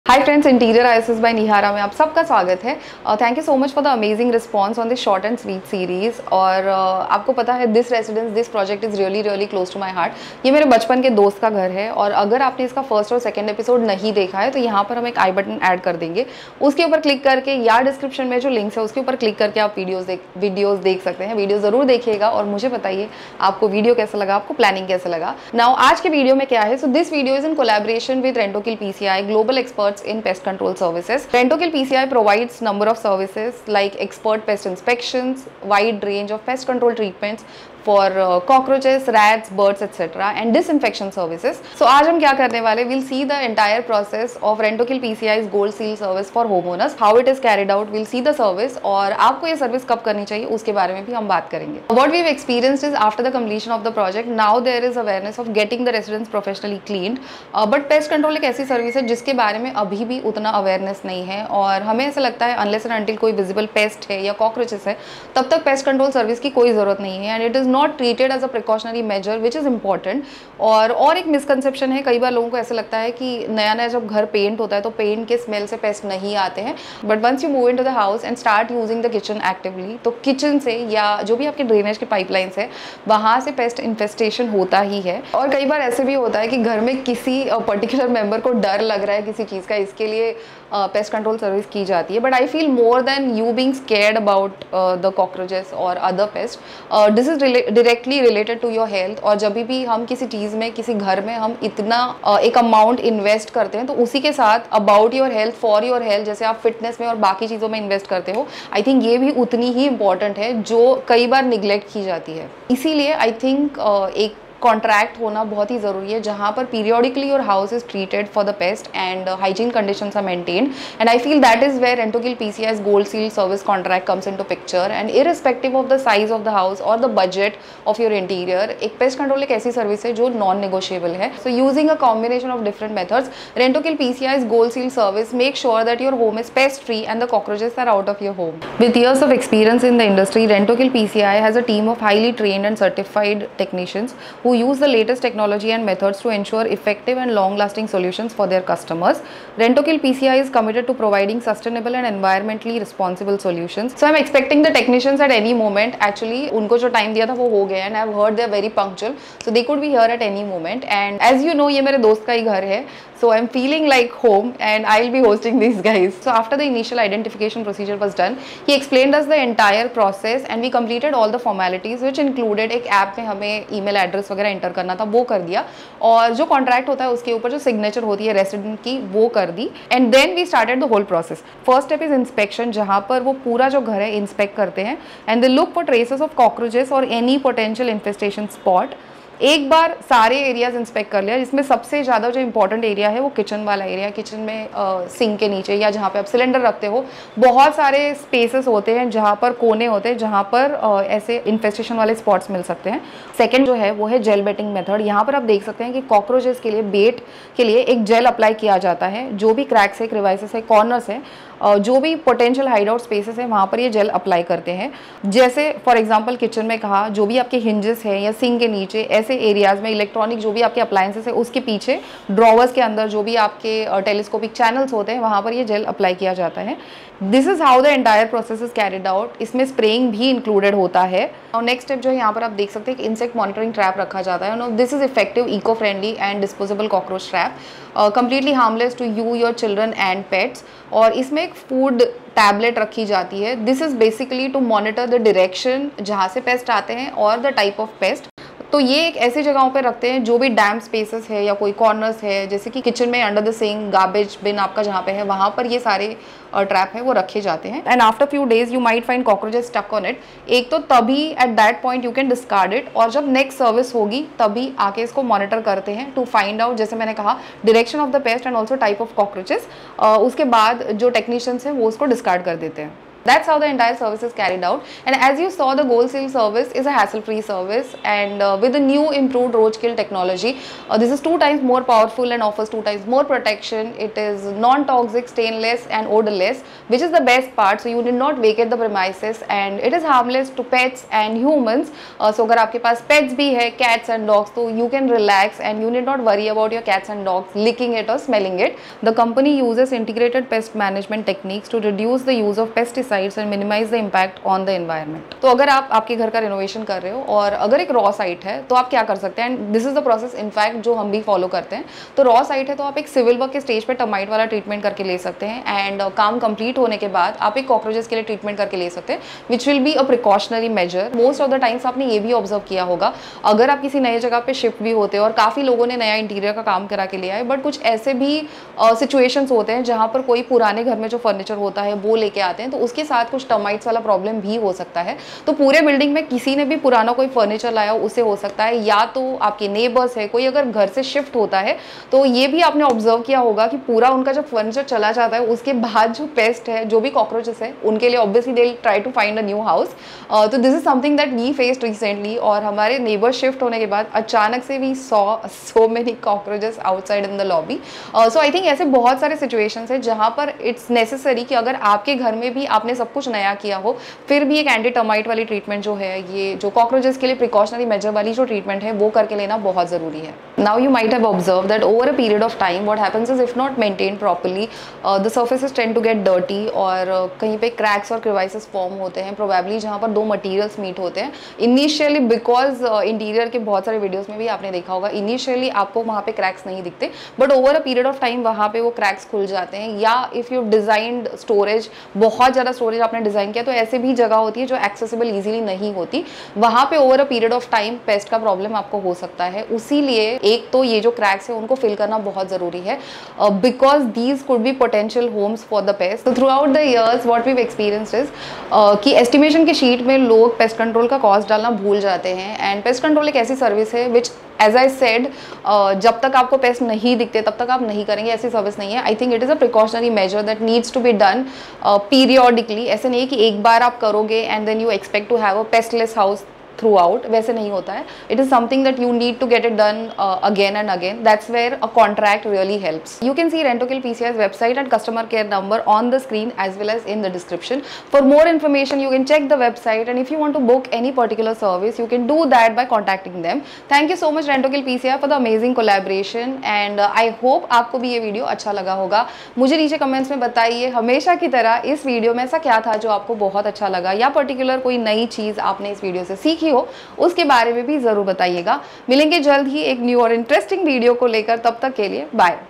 Pest control During Interior design Process | Cockroach control | GoldSeal®️ Service By Rentokil PCI
Hi everyone! In this video we talk about Importance of Pest Control In Interior design of a Home.
to book your service.

00:00 - Introduction
00:25 - What is this video about- Rentokil PCI Collaboration
00:51 -Why is Pest control Important? Myths and Facts...
02:33 - Cockroach Control-Pest control Service by Rentokil PCI Collaboration
03:00 - Step1- Inspection
03:22 - Step2 - Gel Treatment
03:46 - Step3 - Spraying
03-52 - Step4 - Installing Insect Monitoring Trap
04:35 - Details of GoldSeal®️ Service
04:46 - Harmless to Pets and Humans
05:05 - When & Why to do Pest control during Interior Design.
06:09 - Maintenance Tips to minimize pest infestation
07:13 -  To Book Rentokil PCI  Service - Contact number & Website details
07:20 - Subscribe , Share and bye